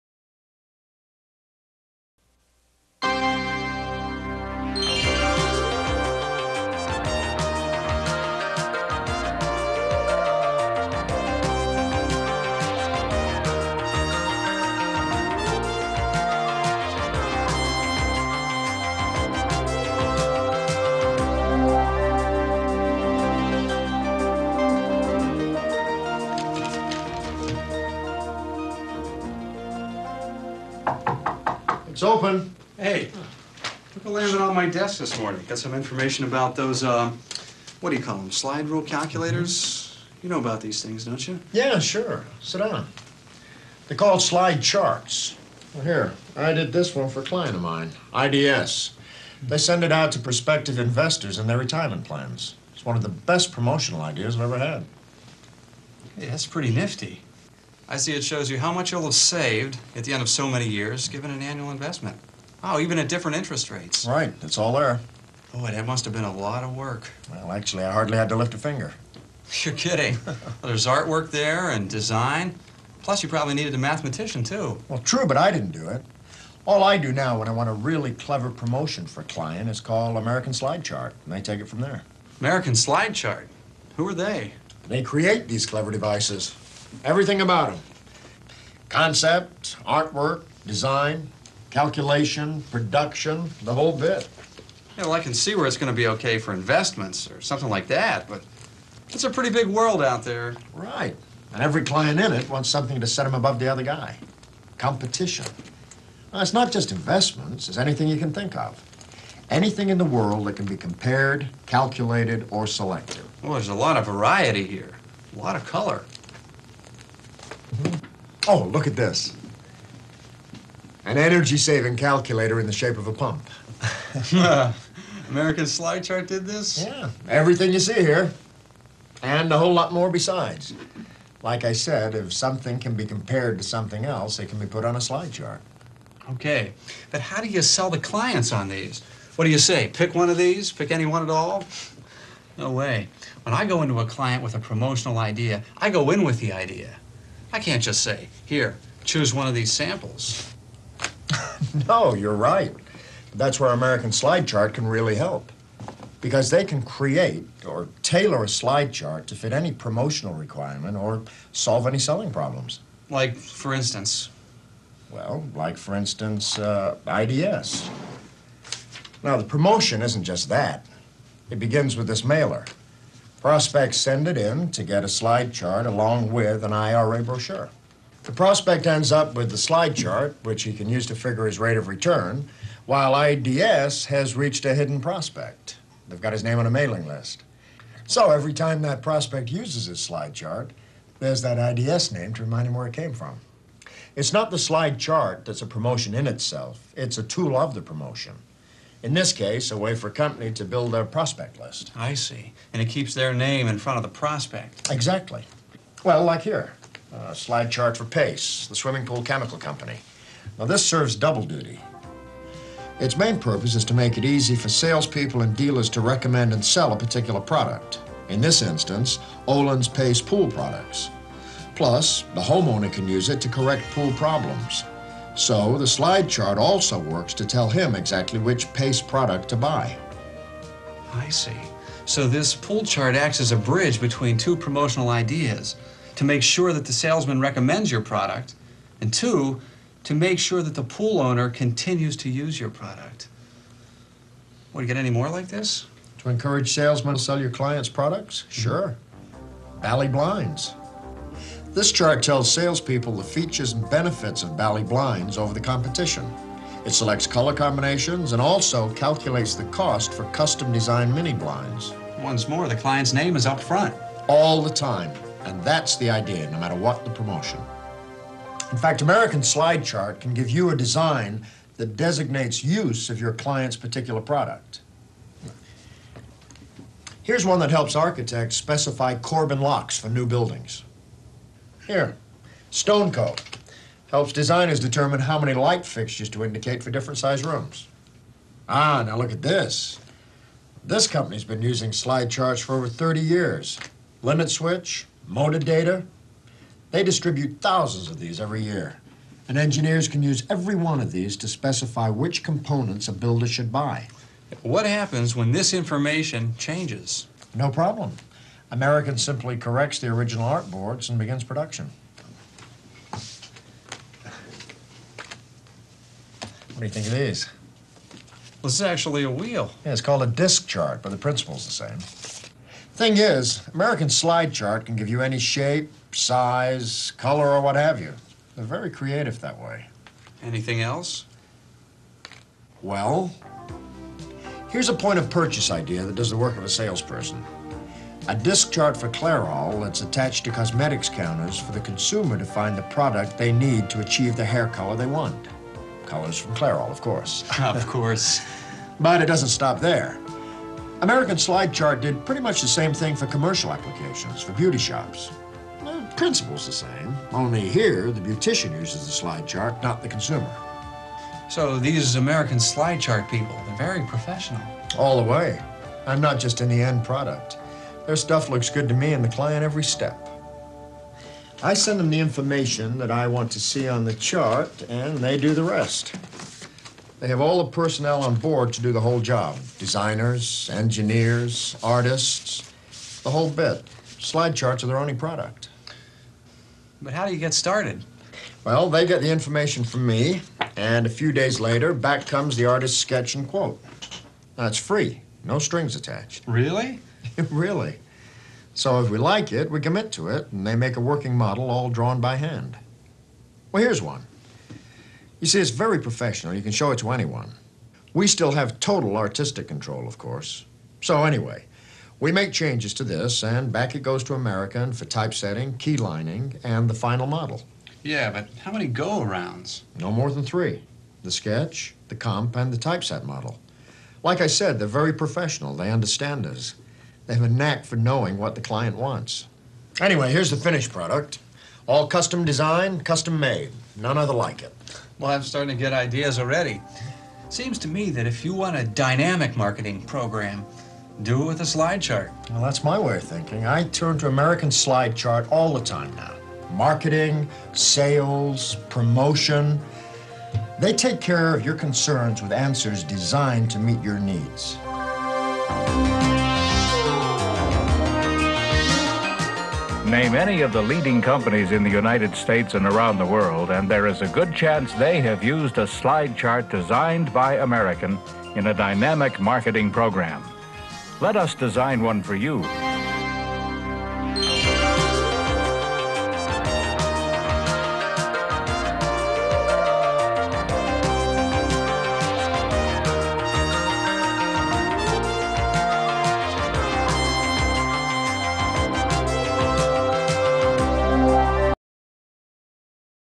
Early John Mahoney Video for American Slide Chart/Perrygraf
Check out this early sales video for American Slide Chart/Perrygraf ... featuring Chicago's very own John Mahoney (of "Frasier" and "Burn Notice") in one of his very first roles!
American Slide Chart/Perrygraf is still here but we have expanded creating not just slide-charts but pop-ups and direct mailers.